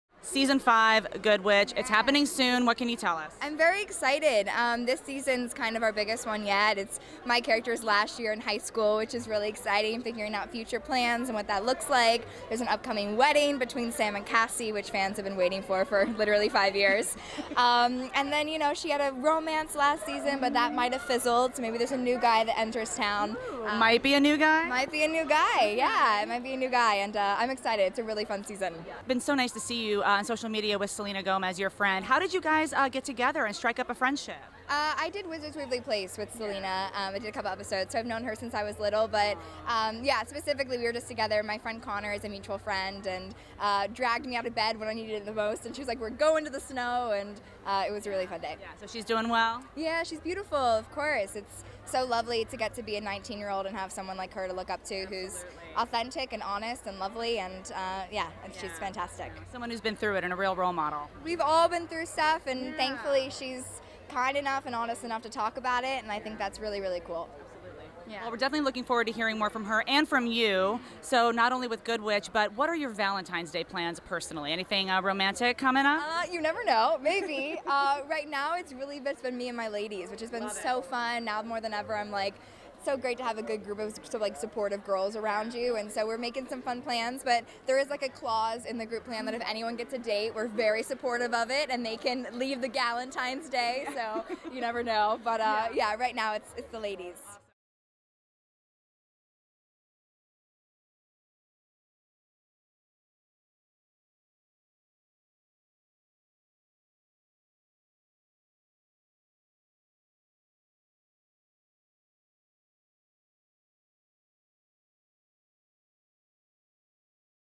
Bailee Madison Gushes Over Her Friendship With 'Beautiful' Selena Gomez: 'She's Fantastic' | Access
"Good Witch" star Bailee Madison tells Access at Hallmark Channel's TCA event about how she's stayed in touch with Selena Gomez since their "Wizards of Waverly Place" days. Find out how their recent and impromptu snow day came to be! Plus, Bailee teases a new romance for her "Good Witch" character, and explains why she's all about her girl squad for Valentine's Day – with a possible catch!